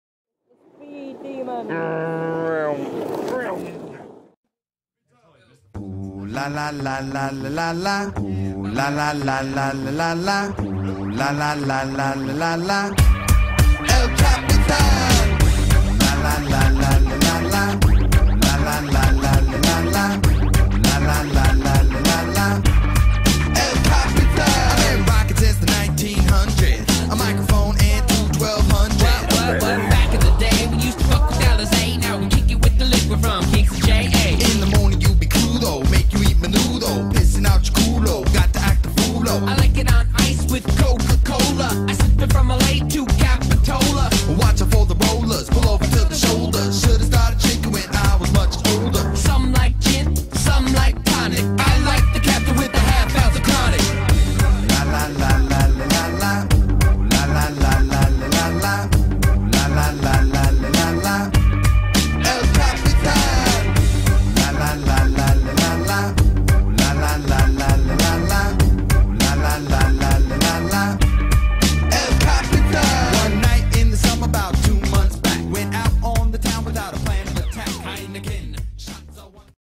PYJAMA/DRESS-UP WEEKEND! Longboarding with friends #6
We decided to celebrate the return to school by all dressing in stupid clothes whilst longboarding and stuff, we hope you enjoy watching as much as we did filming, be sure to tell your friends about us! Comments are always wanted so go ahead and comment anything you want!